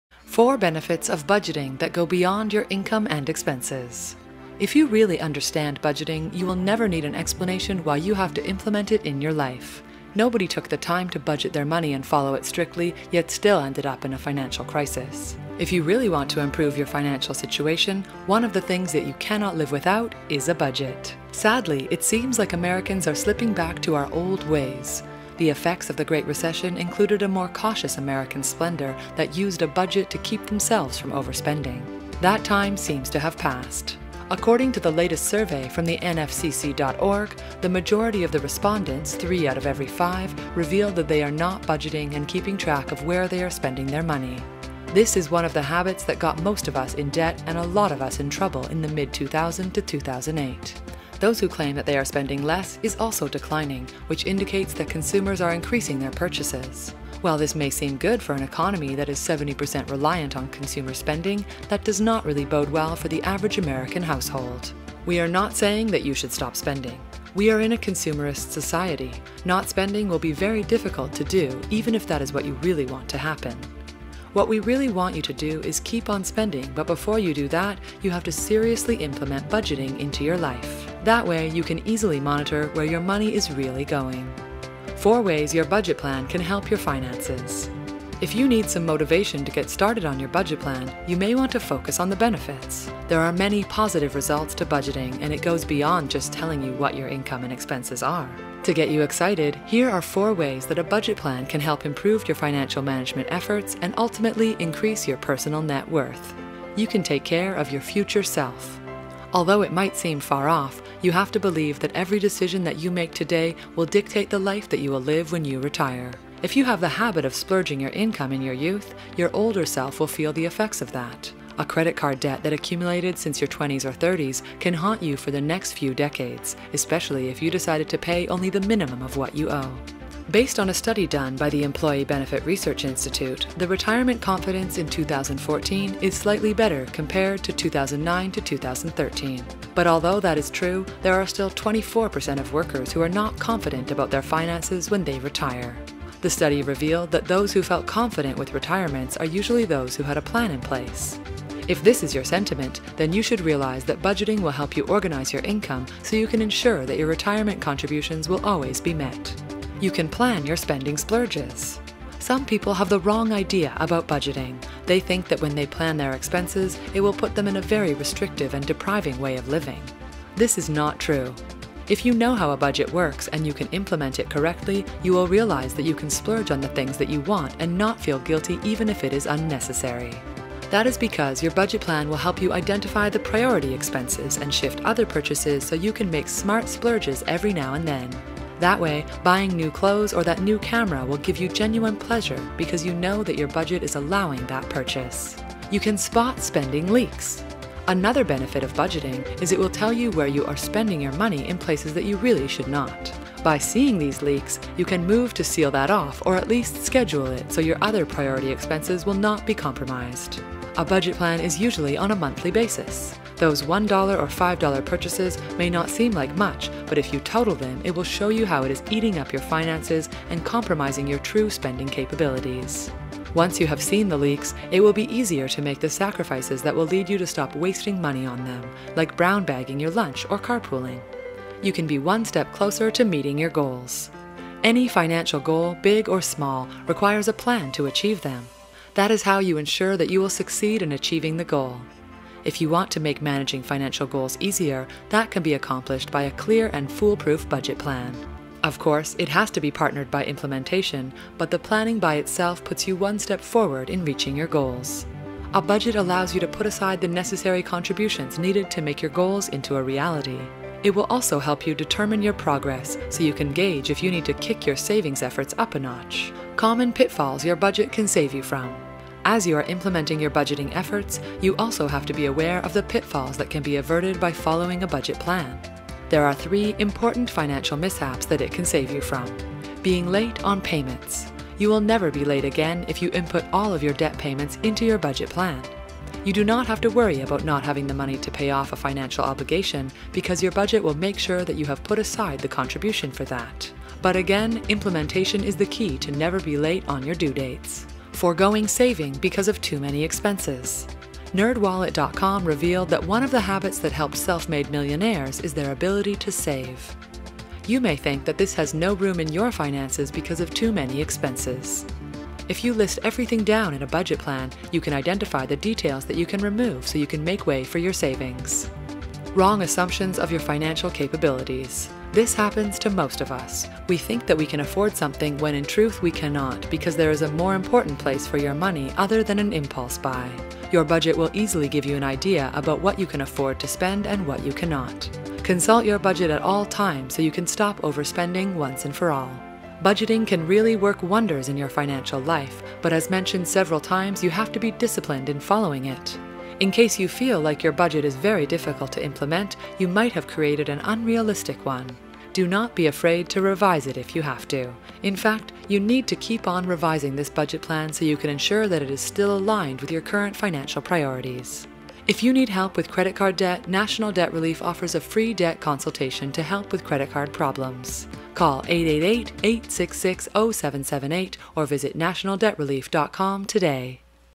4 Benefits of Budgeting That Go Beyond Your Income and Expenses
If you really understand budgeting, you will never need an explanation why you have to implement it in your life. Nobody took the time to budget their money and follow it strictly yet still ended up in a financial crisis. No foolproof budget plan will ever lead to your financial demise. If you really want to improve your finances situation, one of the things that you cannot live without is a budget.

Sadly, it seems like Americans are slipping back to our old ways. The effects of the Great Recession included a more cautious American spender that used a budget to keep themselves from overspending. That time seems to have passed.

According to the latest survey from the NFCC.org, majority of the respondents (3 out of 5), revealed that they are not budgeting and keeping track of where they are spending their money. This is one of the habits that got most of us in debt and in a lot of trouble in the mid 2007 to 2008. Those who claim that they are spending less is also declining – which indicates that consumers are increasing their purchases. While this may seem good for an economy that is 70% reliant on consumer spending, that does not really bode well for the average American household.

We are not saying that you should stop spending. We are in a consumerist society. Not spending will be very difficult to do even if that is what you really want to happen. What we really want you to do is to keep on spending but before you do that, you have to seriously implement budgeting in your life. That way, you can easily monitor where your money is really going.

If you need help with credit card debt, National Debt Relief offers a free debt consultation to help with credit card problems. today.